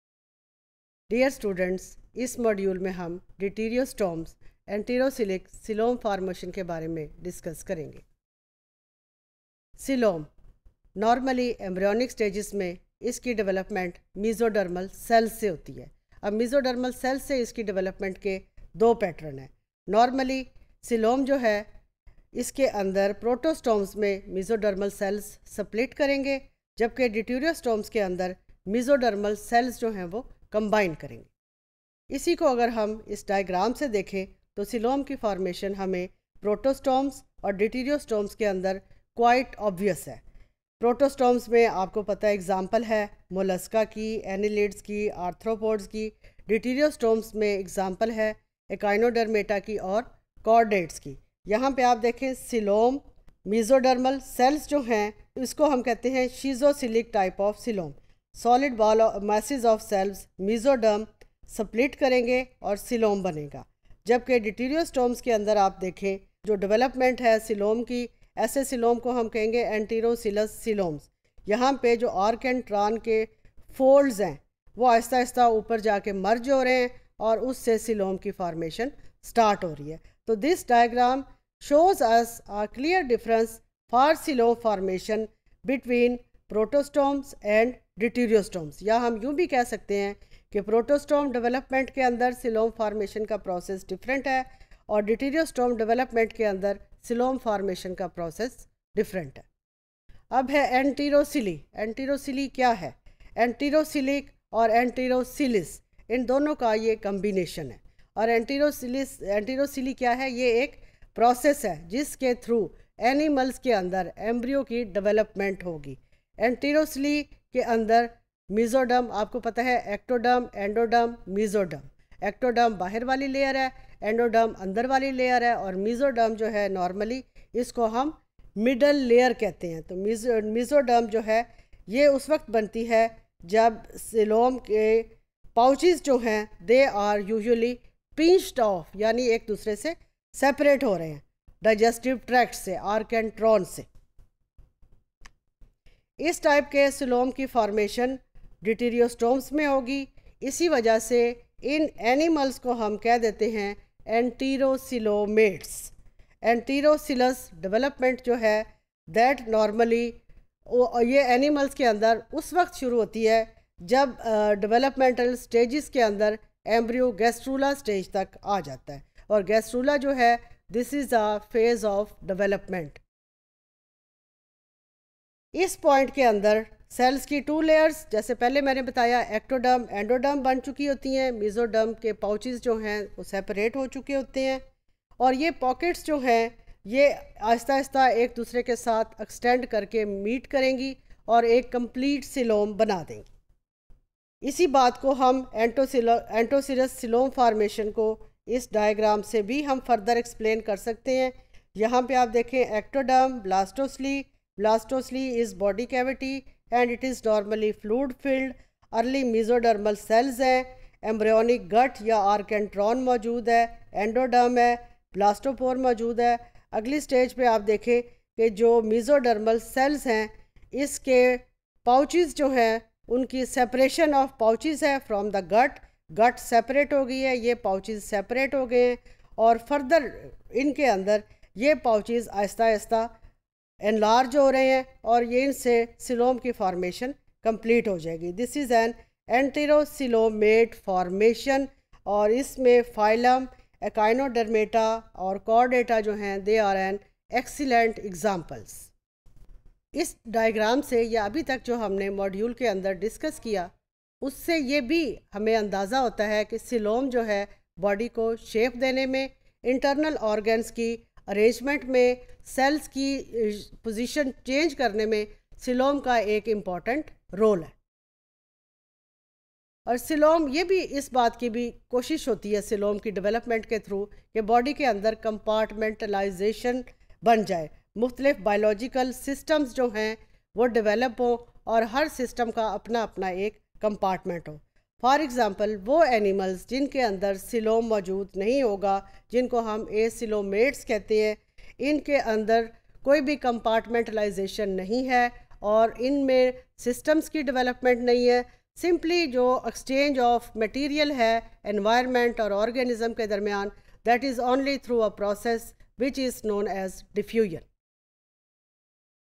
Enterocoelous coelom formation | Animal Diversity Invertebrates (Theory) | ZOO512T_Topic019
Topic019: Enterocoelous coelom formation
by Dr. Farkhanda Manzoor